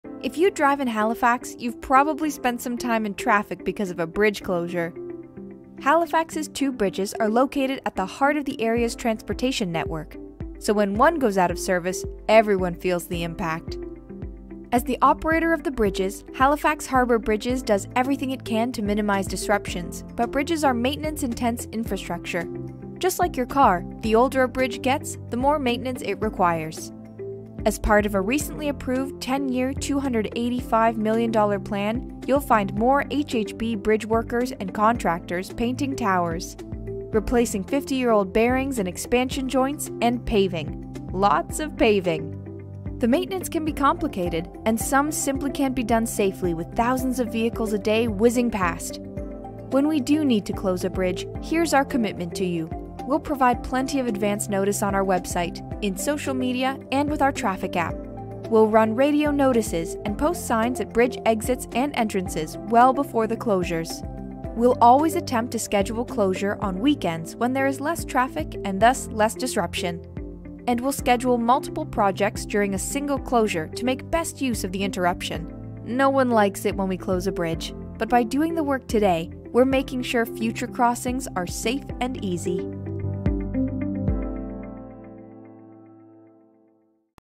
Halifax bridge closures explained
Why are the Macdonald and MacKay Bridges closed so often, and what are bridge crews doing when they are closed? Find out in this short video explaining the work required to keep the aging iconic infrastructure safe.